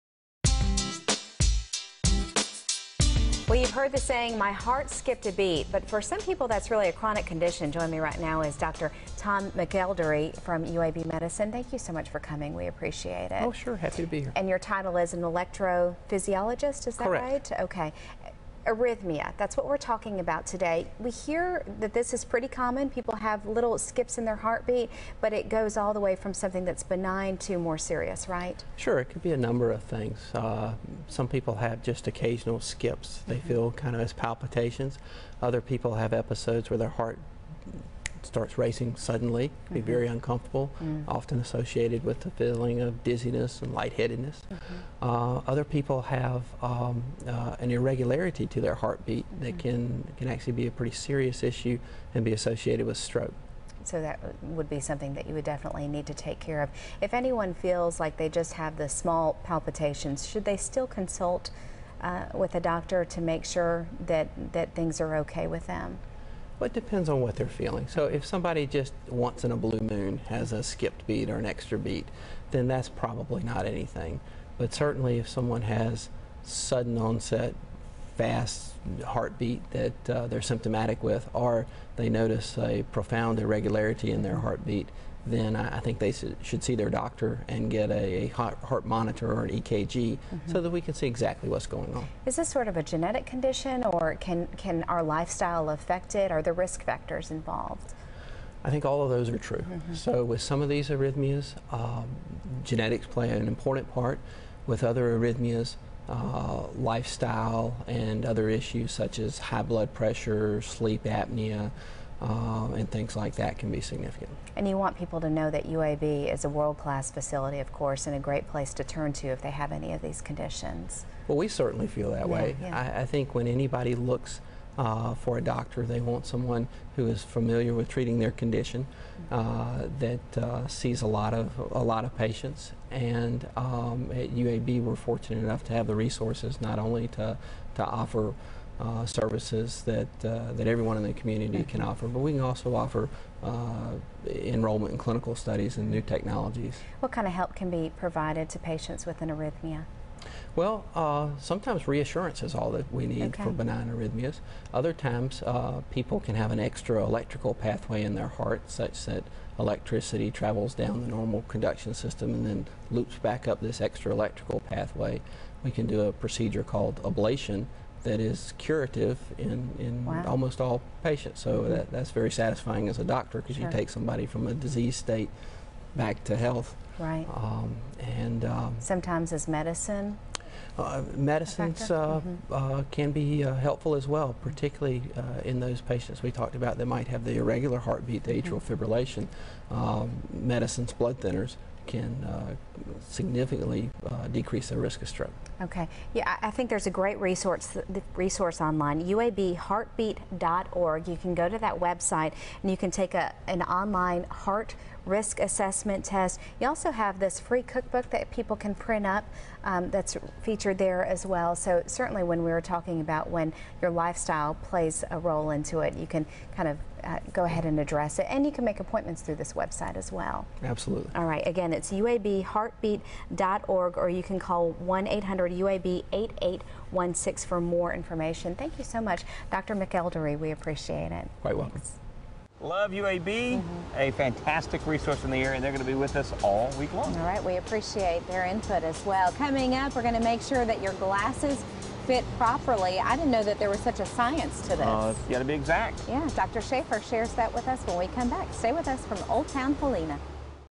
UABMedicine.org | Arrhythmia Risk Factors, Symptoms, & Treatments
If you've ever felt your heart skip a beat, you may have an Arrhythmia. Dr. Tom McElderry, MD, an Arrhythmia specialist at UAB, talks to Daytime Alabama on WVTM-TV, Channel 13, in Birmingham, Ala., about the symptoms and risk factors of Arrhythmia, and what to do if you or a loved one experiences this condition.